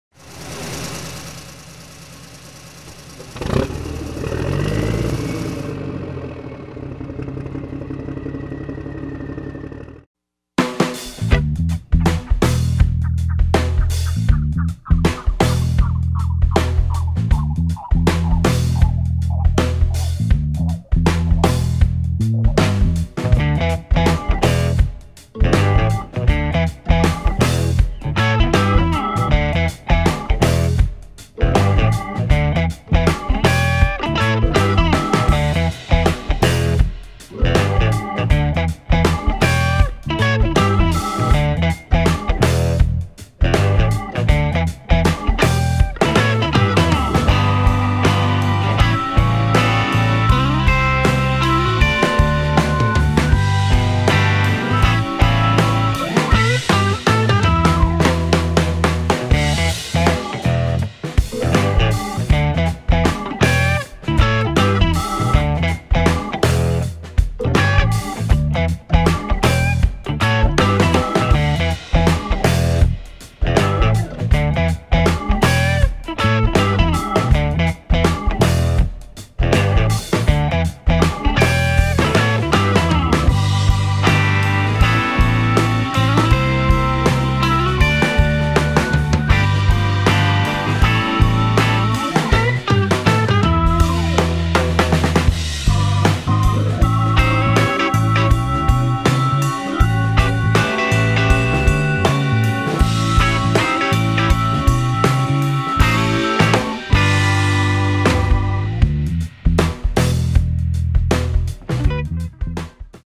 Volvo XC90 - AutoDeal Unboxing
With improved interior, new style and latest safety features, Volvo XC90 is pretty much a desirable, luxury SUV.